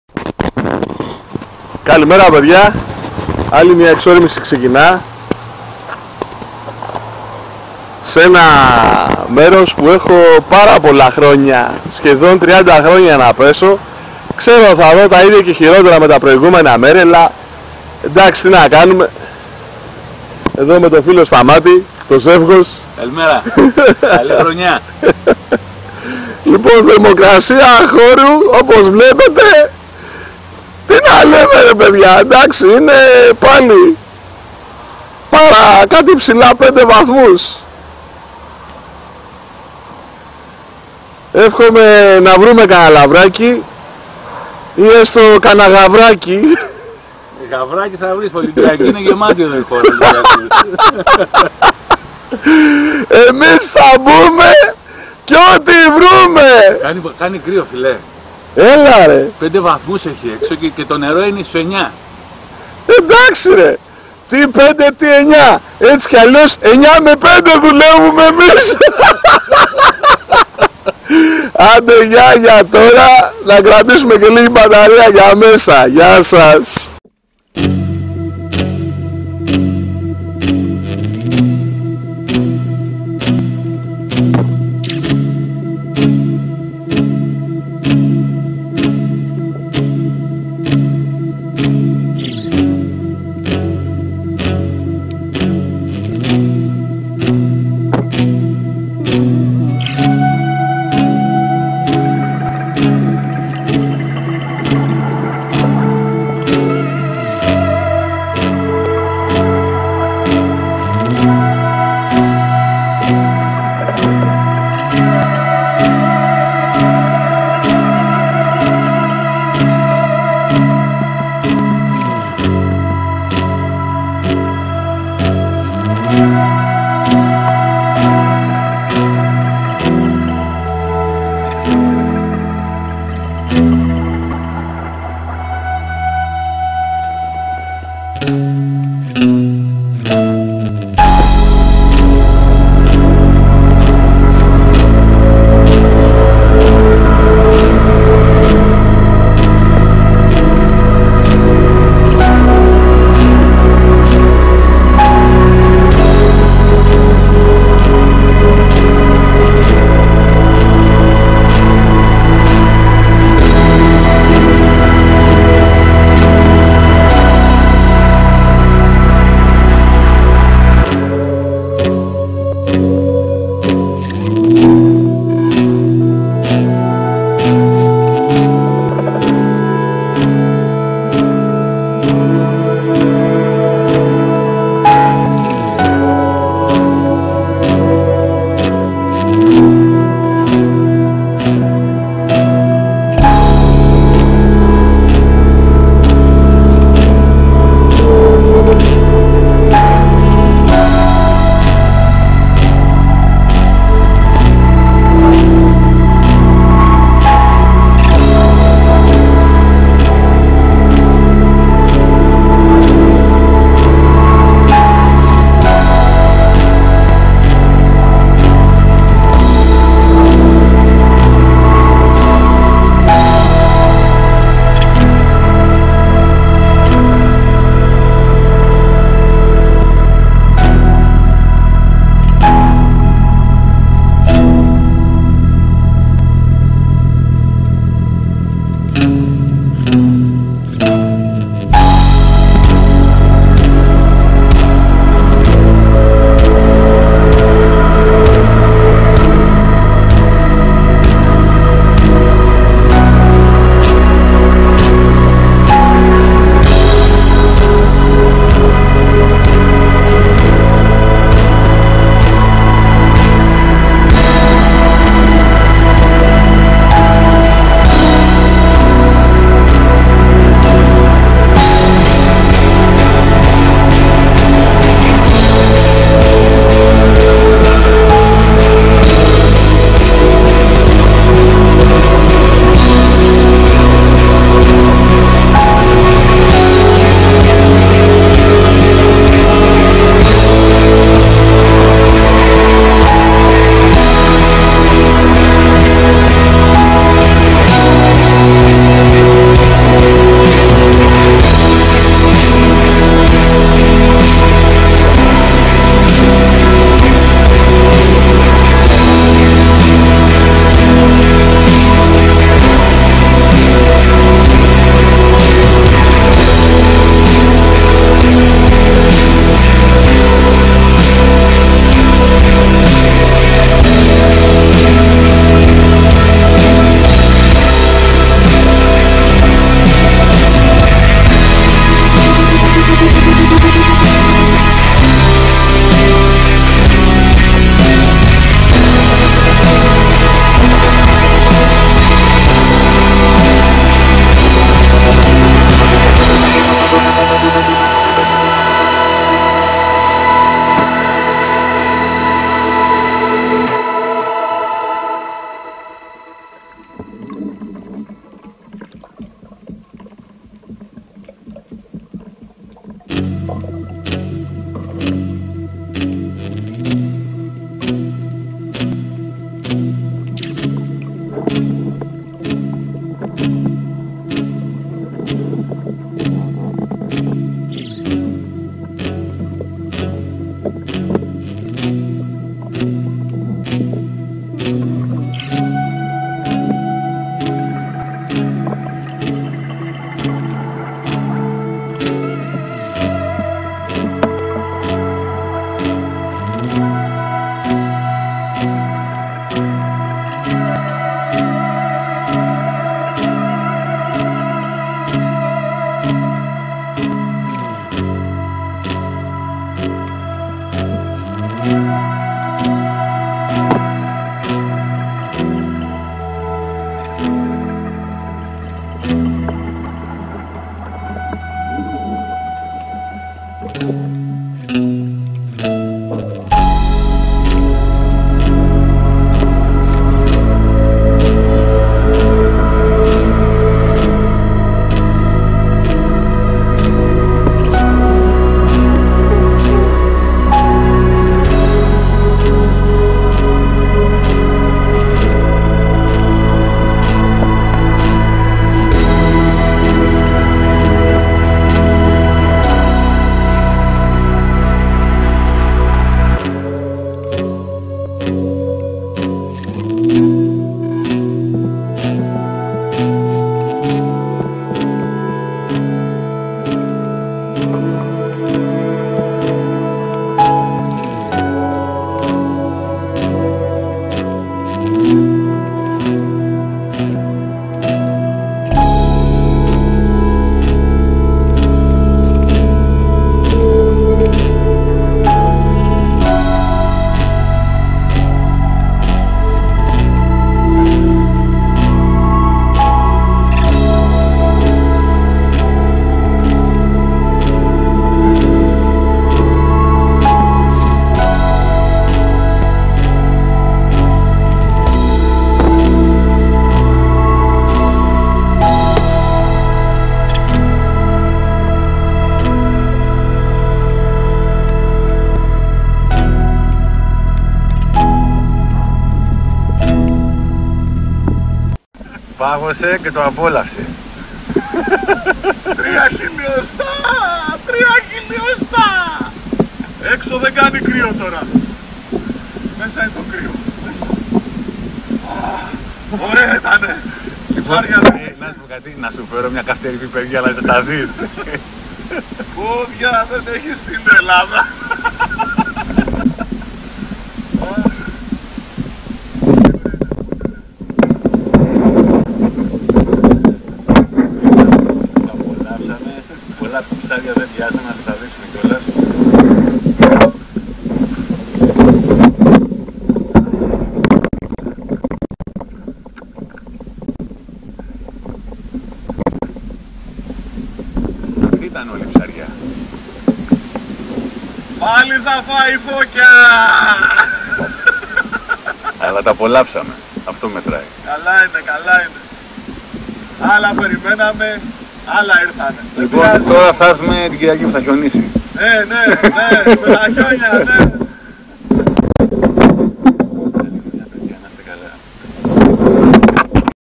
ψαροντουφεκο 4 1 2017 spearfishing
καλή χρονιά σε όλους πήγαμε με το Σταμάτη για να δοκιμάσουμε τη στολή του καινούρια 5χιλ. VS 3χιλ. της παλαιάς δικιάς μου λογικό με τη θερμοκρασία αυτή 9C να χάσω αλλά πάλεψα στα ίσα.
καιρός το πρωί παγωνιά και υγρασία πάχνη πάνω από τη θάλασσα μετά ήλιος.
θερμοκρασία έξω το πρωί 4-5c μετά 14c μέσα 9c
άνεμος πρωί άπνοια μετά 2β Ν
παρουσία ψαριών απουσία αξιόλογων στόχων(Λακκάκια,Τσιπούρες)
μέρος καλός τόπος για όλα αλλά θέλει καλό ψάξιμο θέση 2
βάθος βουτιών από 1-8μ.
ΚΑΛΕΣ ΘΑΛΑΣΣΕΣ ΣΕ ΟΛΟΥΣ ΜΑΣ!!!